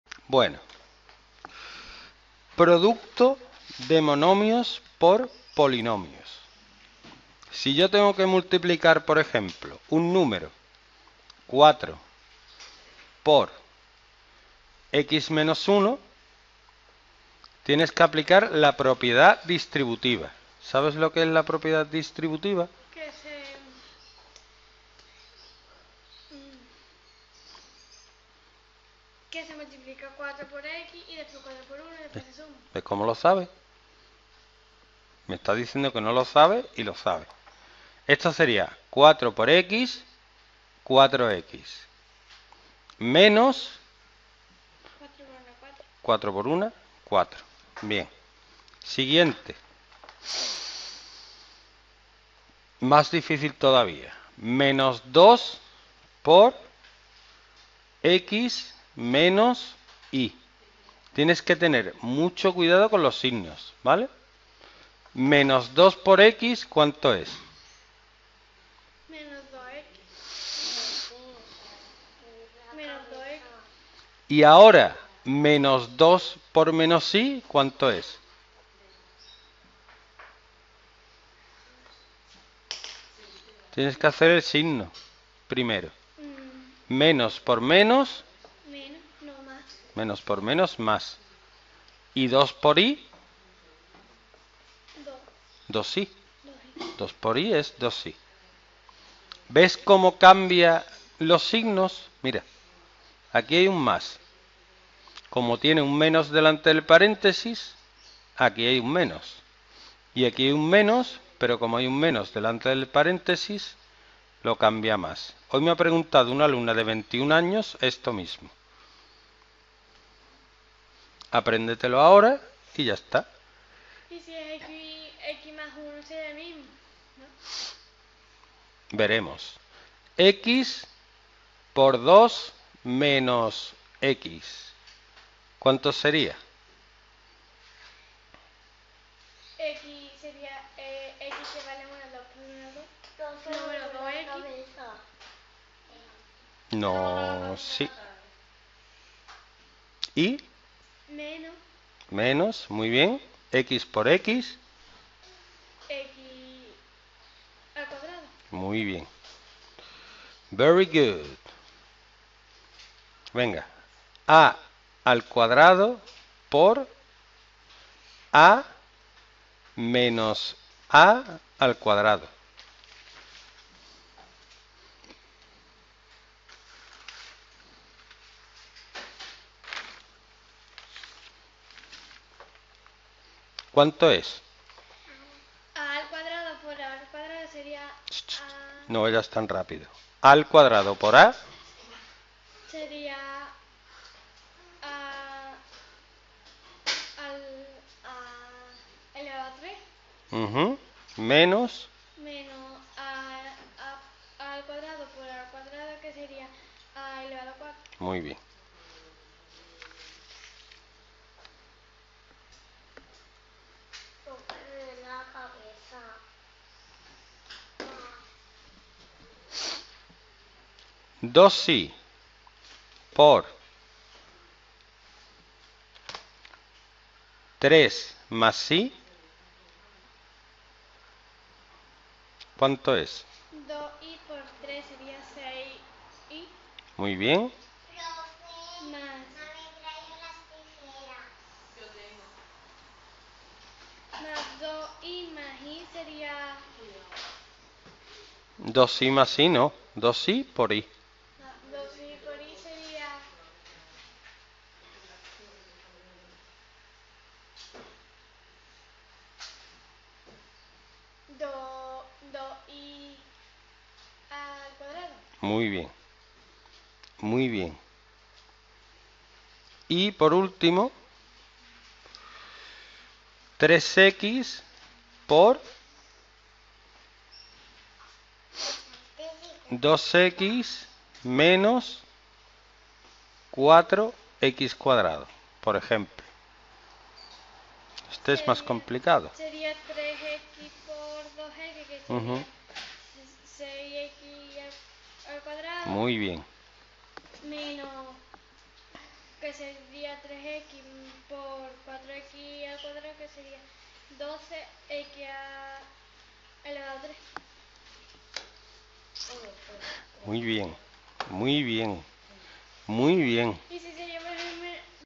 Producto de monomios por polinomios Matemáticas 1º ESO Academia Usero Estepona
Practicamos la multiplicación de monomios por polinomios usando la propiedad distributiva. Este conocimiento será muy útil para la posterior realización de ecuaciones.